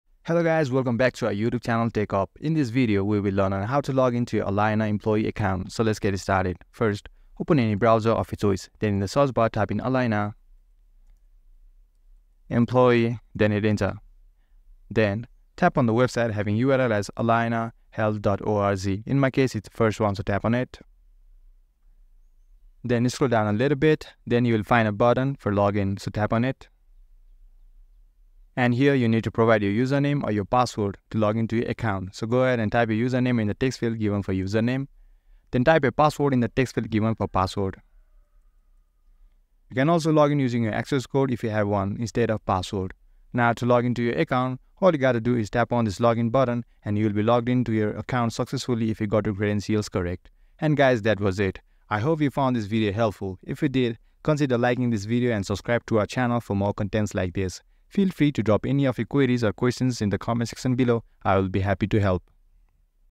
How to Login to Allina Employee Account, 2024?
In our detailed guide, 'How to Login to Allina Employee Account,' we’ll walk you through the steps to access your employee resources quickly and securely. Whether you need to view your schedule, benefits, or other important information, this guide will help you get started. Join us as we demonstrate the login process. Don’t miss out – learn how to log in to your Allina employee account now!